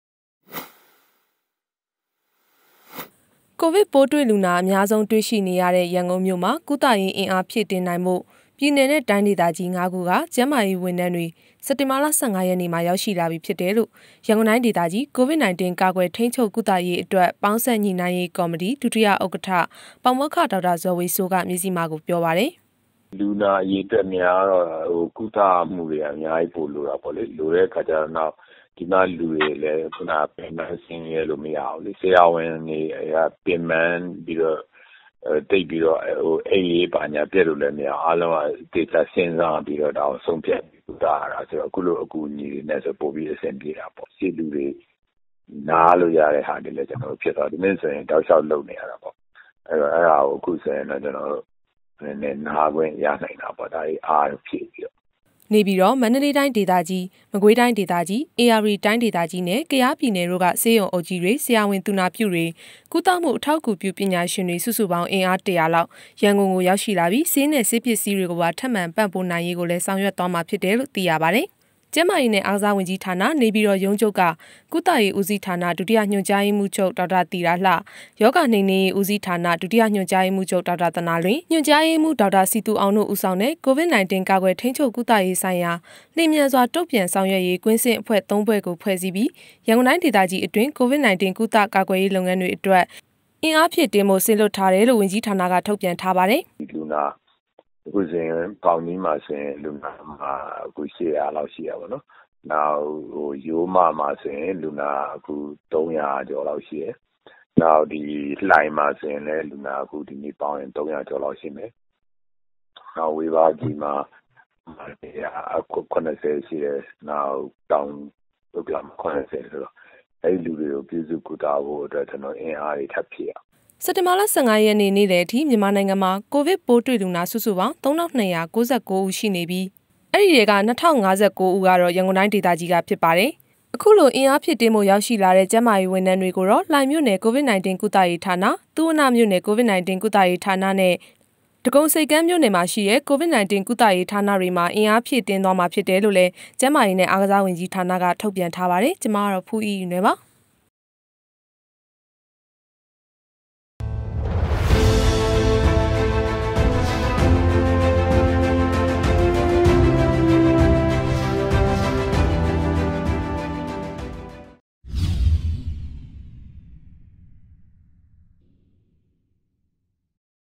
ပြည်နယ်နှင့် တိုင်းဒေသကြီး ၅ ခုမှ ကျန်းမာရေးဝန်ထမ်းများ ရန်ကုန်သို့ လာရောက်အင်အား ဖြည့်တင်း
ကိုဗစ်ပိုးတွေ့လူနာအများဆုံးတွေ့ရှိနေရသည့် ရန်ကုန်မြို့တွင် ကုသရေး အင်အားဖြည့်တင်းရန် ပြည်နယ်နှင့်တိုင်းဒေသကြီး ငါးခုမှ ကျန်းမာရေးဝန်ထမ်းများ စက်တင်ဘာလ ၁၅ ရက်နေ့တွင် ရောက်ရှိလာပြီဖြစ်ကြောင်း ရန်ကုန်တိုင်းဒေသကြီး COVID-19 ကာကွယ်ထိန်းချုပ်ကုသရေး အတွက် ပေါင်းစပ်ညှိနှိုင်းရေးကော်မတီ ဒုတိယဥက္ကဋ္ဌ-၂ ပါမောက္ခဒေါက်တာဇော်ဝေစိုးက မဇ္ဈိမကို ပြောသည်။

တည်းဖြတ်  / တင်ဆက် - ဖူးအိအိနွယ်